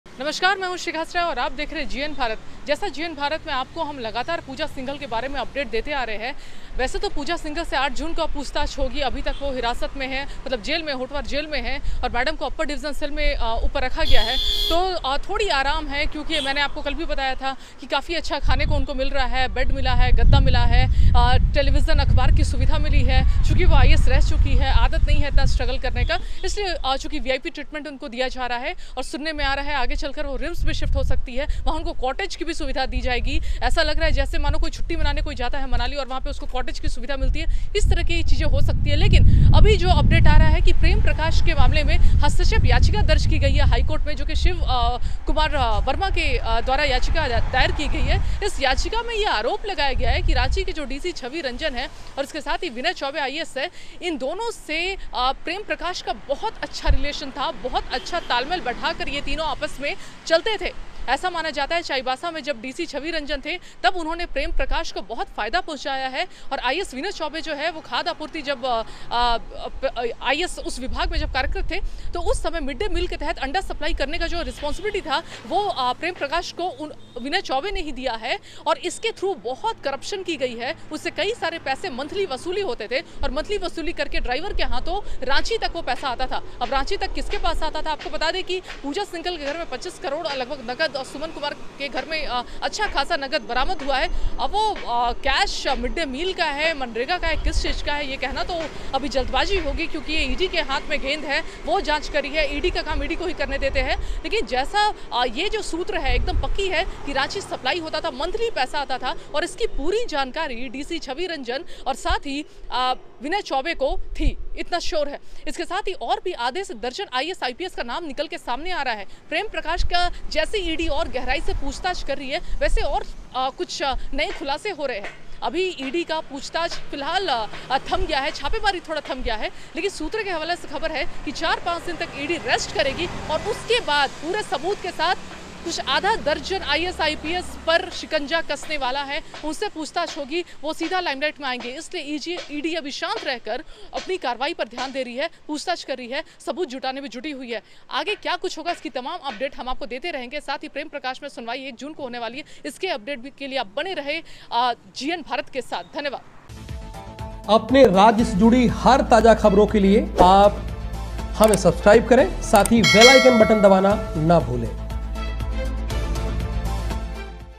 पूजा सिंघल मामले में अब नहीं बचेगा कोई, पूजा के भ्रष्टाचारी जाल में अब सबका होगा जलप्रलय !
पूजा_सिंघल मामले में अब नहीं बचेगा कोई, पूजा सिंघल मामले में सबसे बड़ा